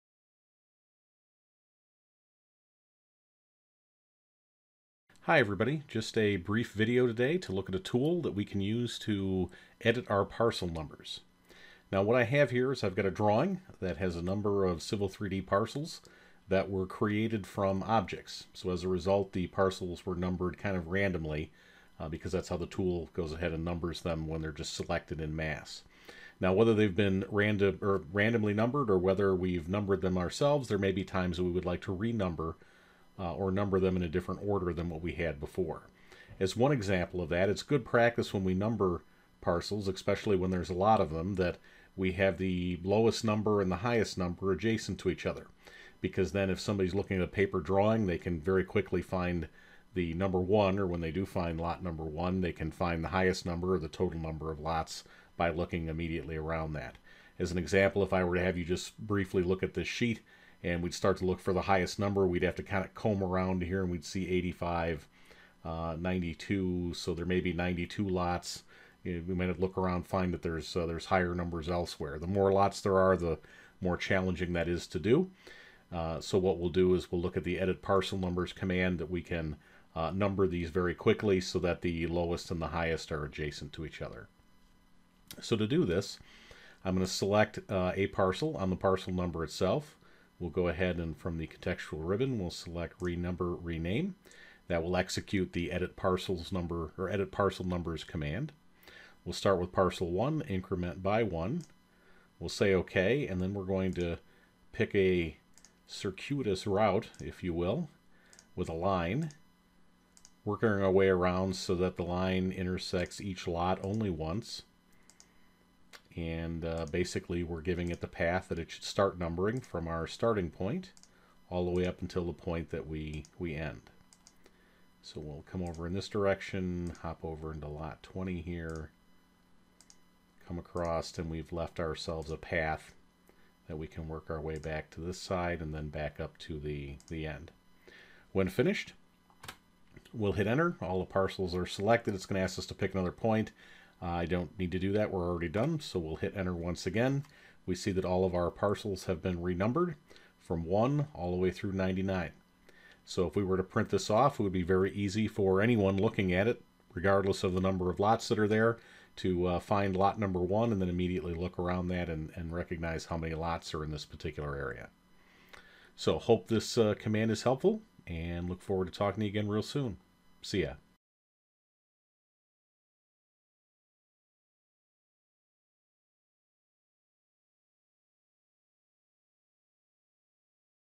Renumbering Parcels in Civil 3D
In this brief (3 min) video, I show how the "EditParcelNumbers" command in AutoCAD Civil 3D can be used to quickly and easily renumber Civil 3D parcel objects.